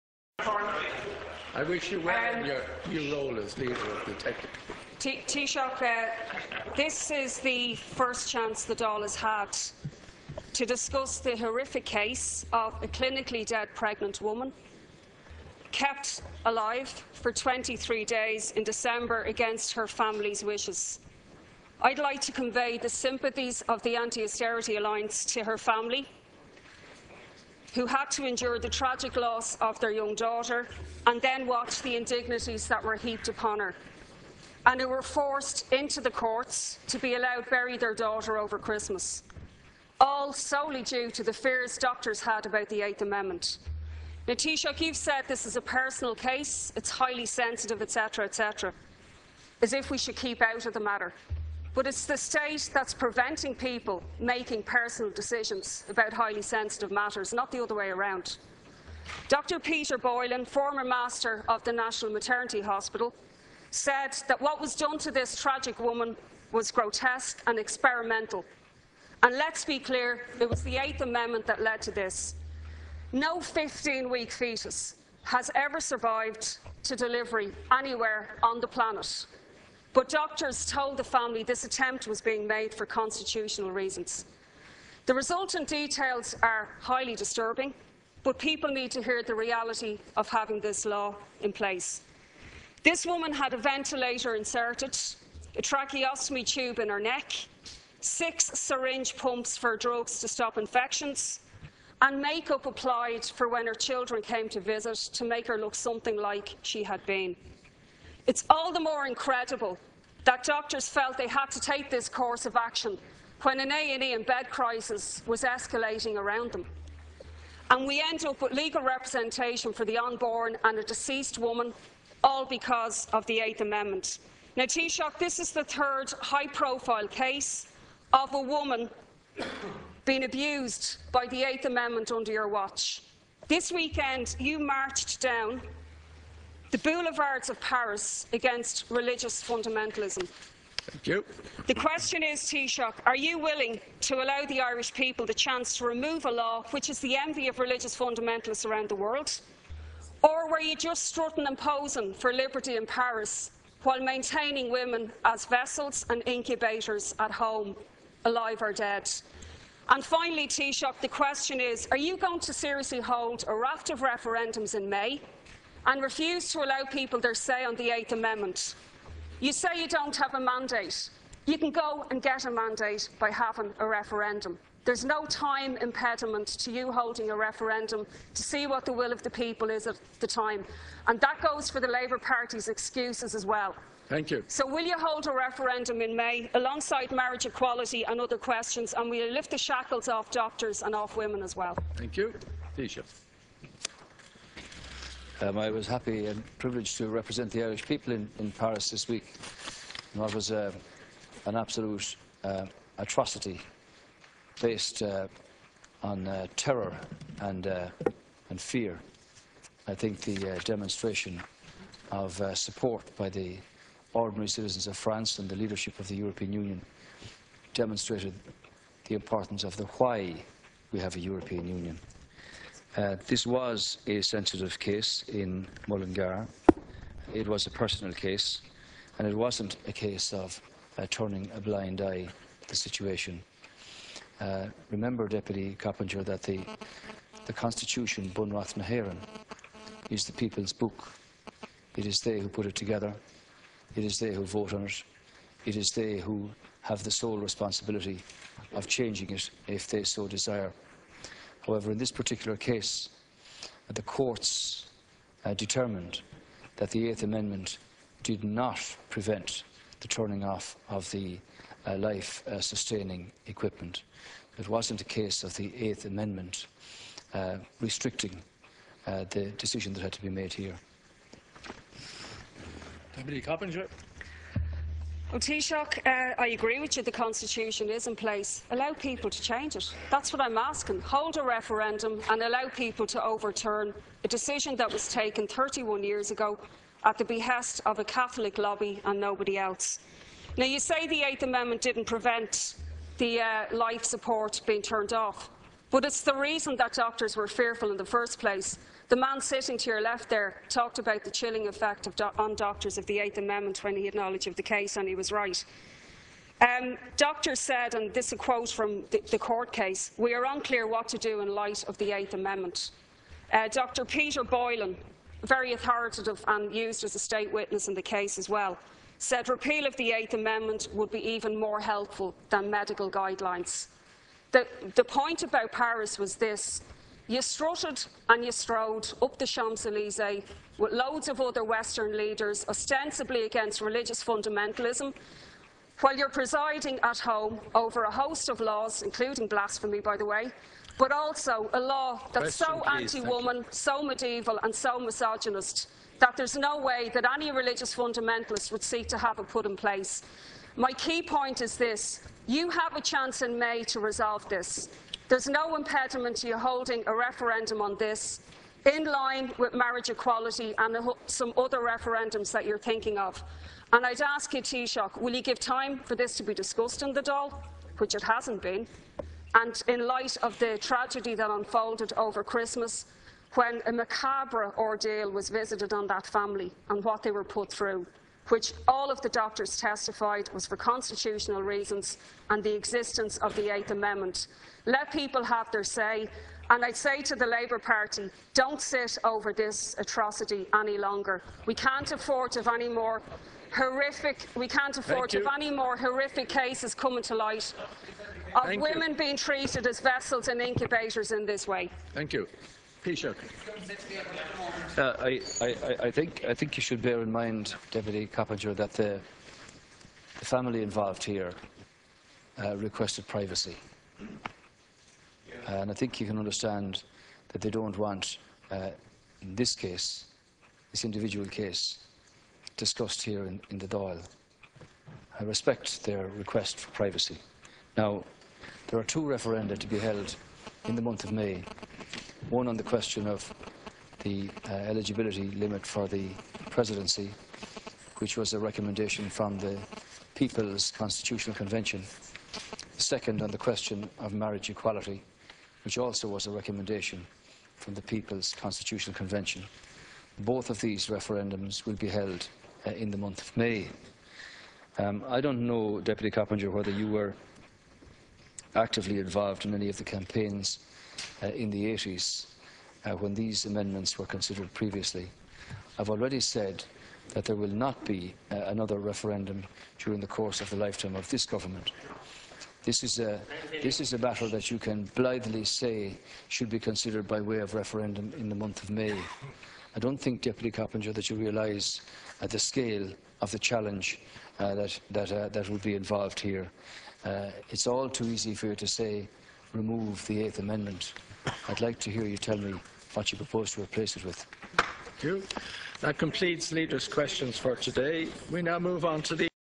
Ruth Coppinger TD: "You march in Paris, yet uphold laws any religious fundamentalist would envy"
Ruth Coppinger TD raises the case of a clinically dead pregnant woman being kept alive against her families wishes and calls for a referendum to be held in May to repeal the 8th amendment